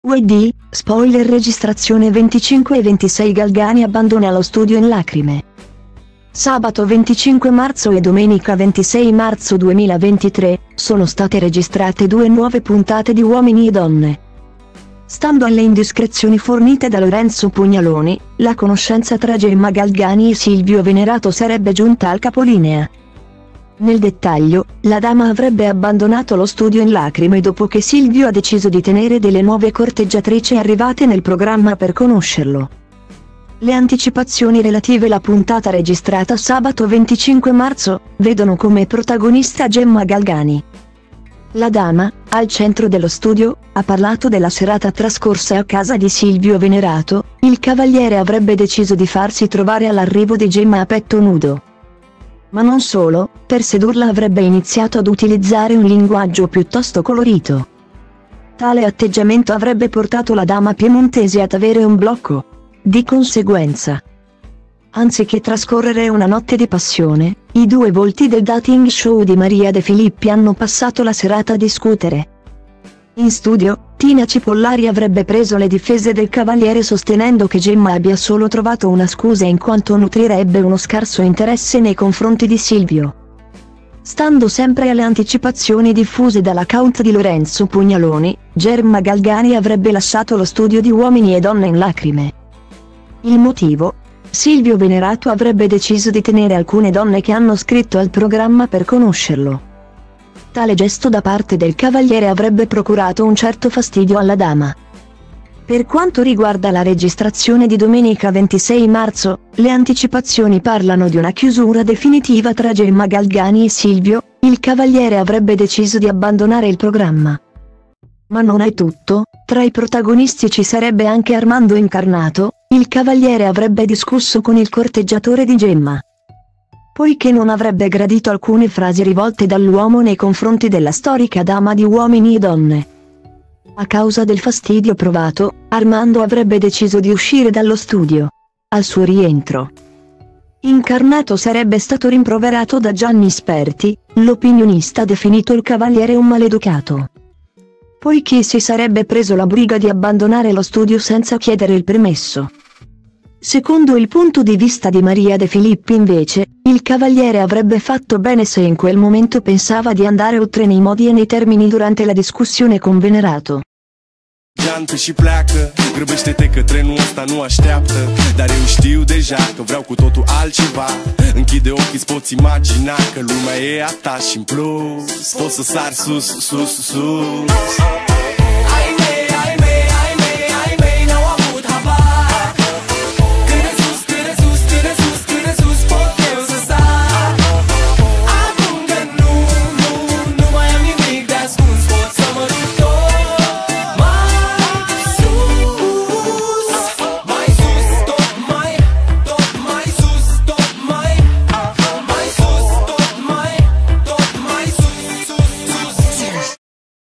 Uomini e Donne, spoiler registrazione 25 e 26 Galgani abbandona lo studio in lacrime
Sabato 25 marzo e domenica 26 marzo 2023, sono state registrate due nuove puntate di Uomini e Donne.Stando alle indiscrezioni fornite da Lorenzo Pugnaloni, la conoscenza tra Gemma Galgani e Silvio Venerato sarebbe giunta al capolinea.Nel dettaglio, la dama avrebbe abbandonato lo studio in lacrime dopo che Silvio ha deciso di tenere delle nuove corteggiatrice arrivate nel programma per conoscerlo.